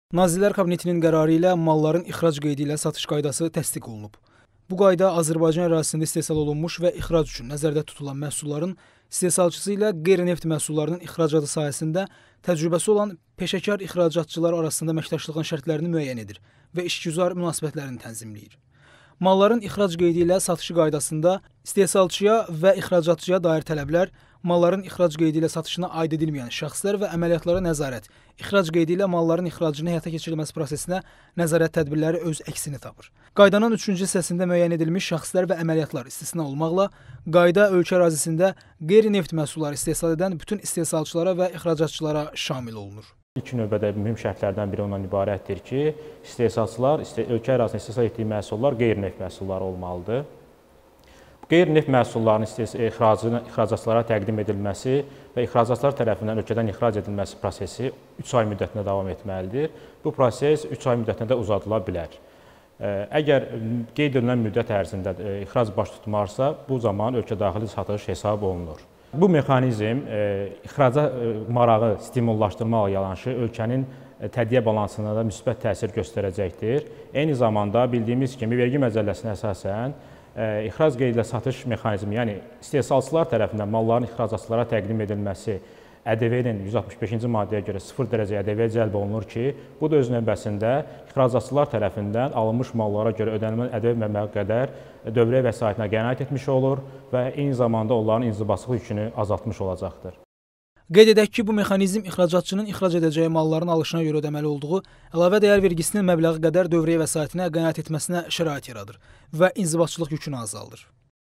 İxracatçıların inzibatçılıq yükü azalır
Malların ixrac qeydi ilə satışı qaydası təsdiq edilib